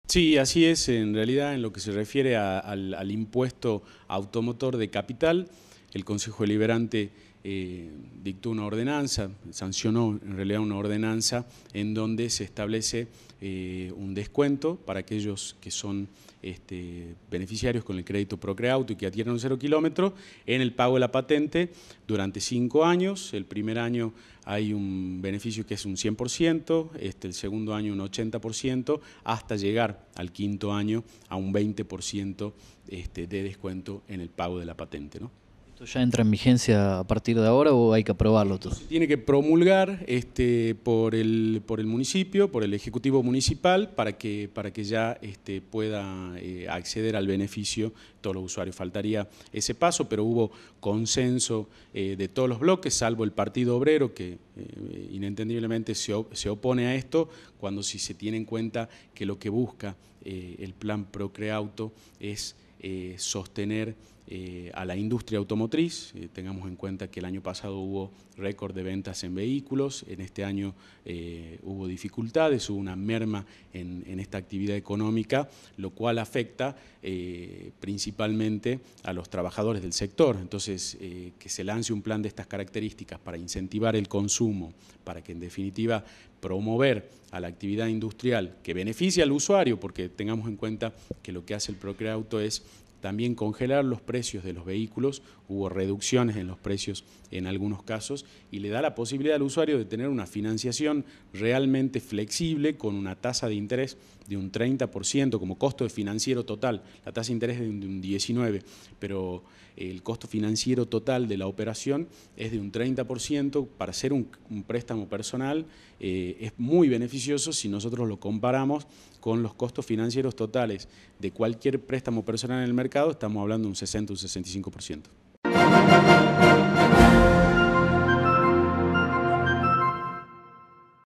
PlAN PROCREAUTO-TV DOS SALTA
Santiago Godoy, Secretario de la Secretaría de Defensa del Consumidor de Salta, habla sobre la importancia de nuevos beneficios que otorga el plan Procreauto. (DOS EN LA NOTICIA)